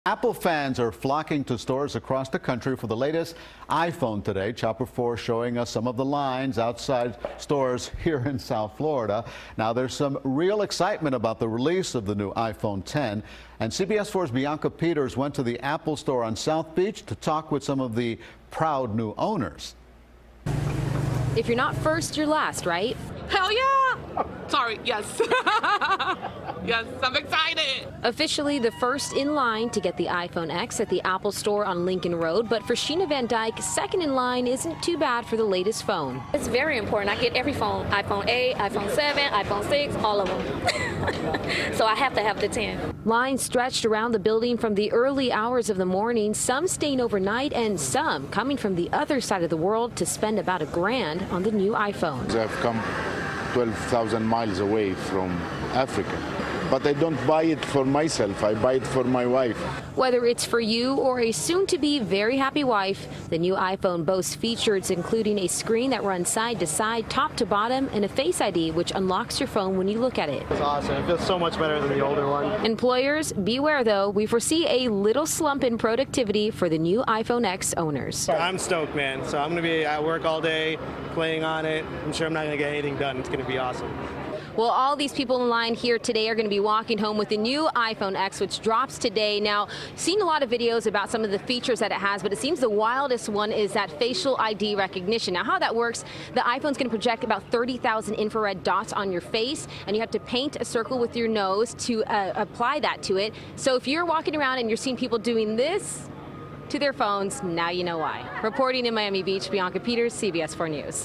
Line Stretches Around South Beach Apple Store For New iPhone X
Hundreds stood behind her in line to get the iPhone X at the South Beach Apple Store on Lincoln Rd.